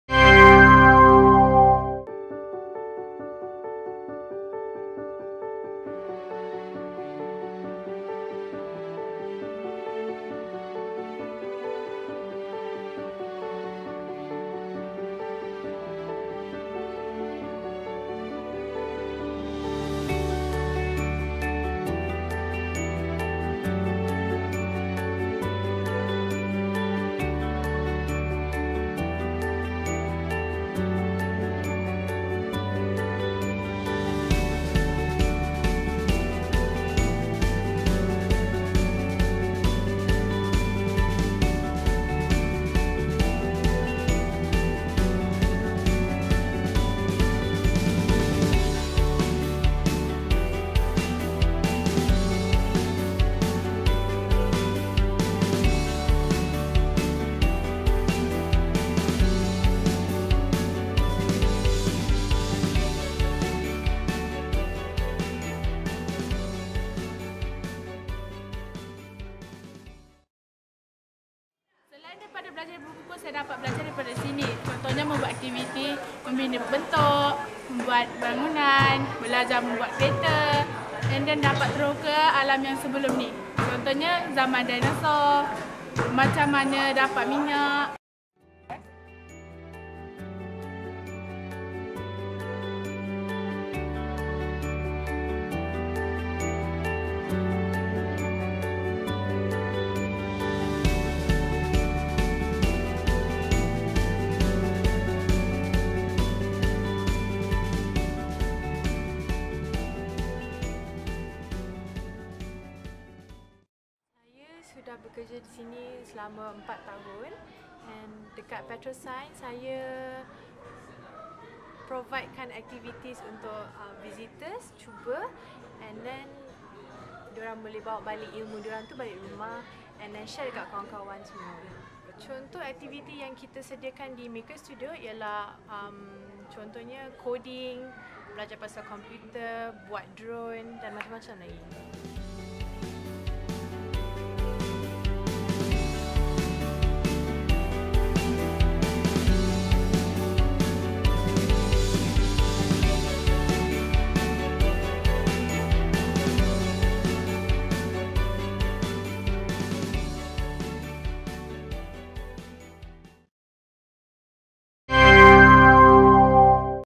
Mengembara ke dunia sains , teknologi di Petrosains
KUALA LUMPUR 30 Jun - Interaktif, berpengetahuan dan menghiburkan. Itu yang dapat disimpulkan apabila berkunjung ke Petrosains di pusat beli-belah Suria KLCC, di sini.